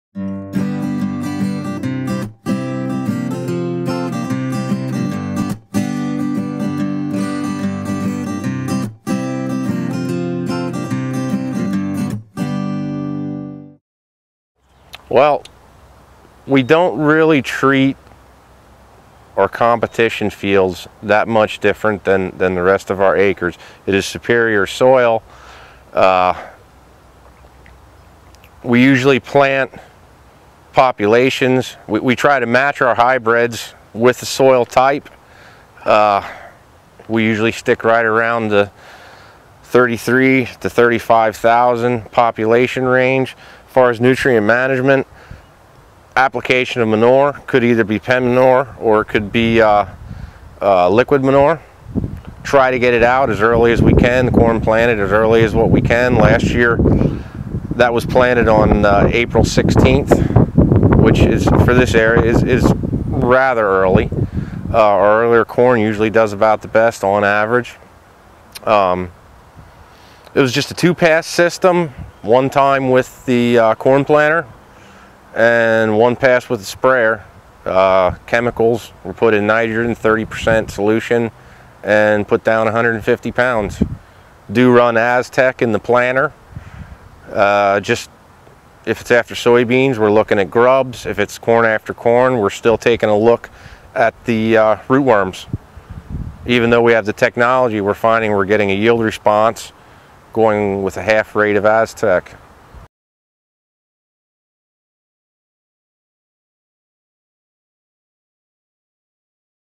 No-Tiller Breaks 300 Bu:A In 2012; Shares Details Of Successful Year
David Wolfskill, Wernersville, Pa., no-tiller, registered second place in the 2012 national corn yield contest with 307 bushels per acre. In this video, he explains how he achieved this success.